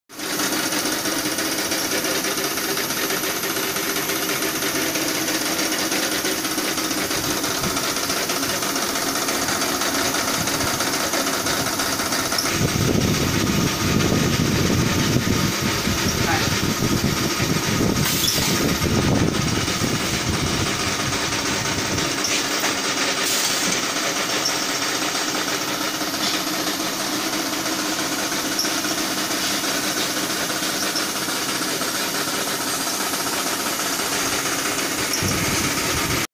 Diaper Huggies Bag pouch seal and cut pumematic machine
Daiper Huggies pouch sealing with cutting  machine
Royal Pack Industries mumbai